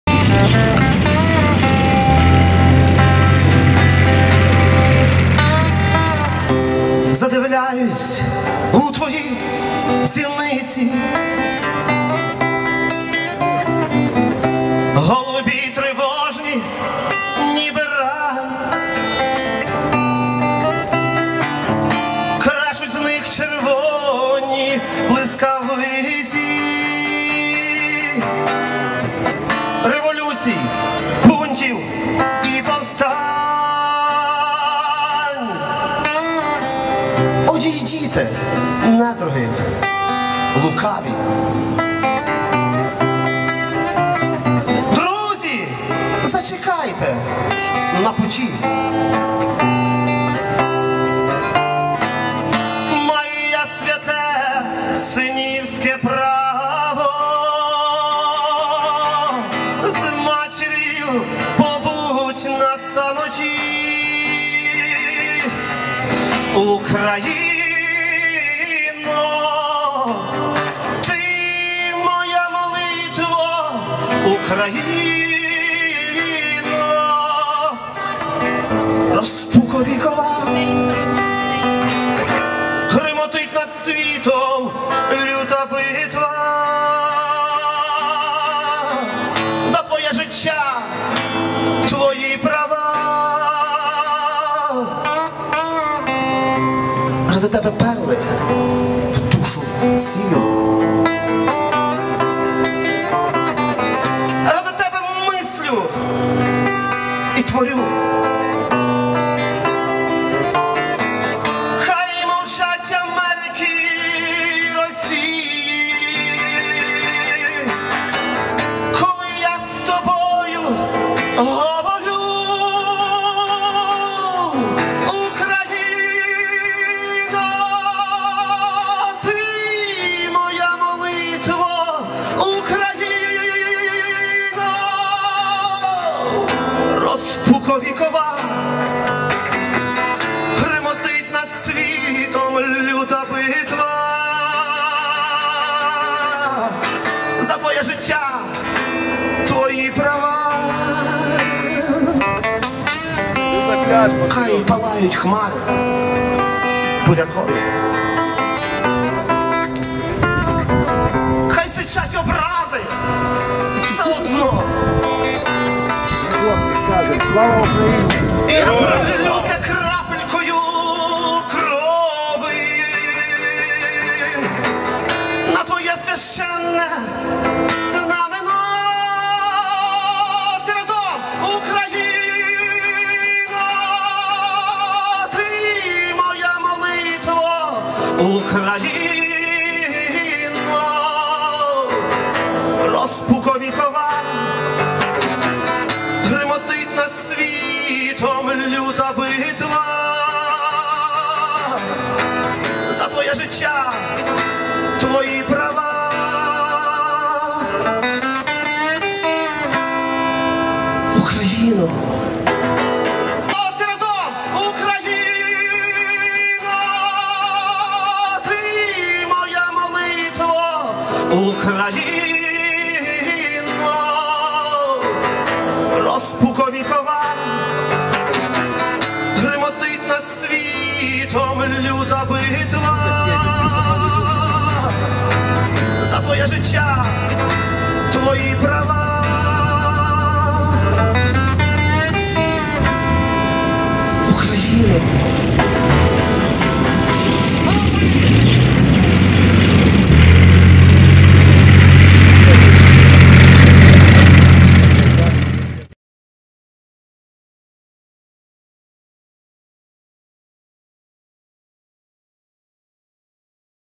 Misija Ukraina. Kijevas - Chersonas. Kariai.
2014 balandžio 16-20 dienos.
Misijos dalyviai:
Vilma Valiukevičienė
Ivanas Dorošas
Andrius Almanis
Vytautas Riaubiškis.
Pasitiko ir lydėjo - Natalija Bimbiraitė (Ukraina)
Nuotraukos Natalijos Bimbiraitės ir kitų misijos dalyvių.
Vaizdo įrašai Vytauto Riaubiškio.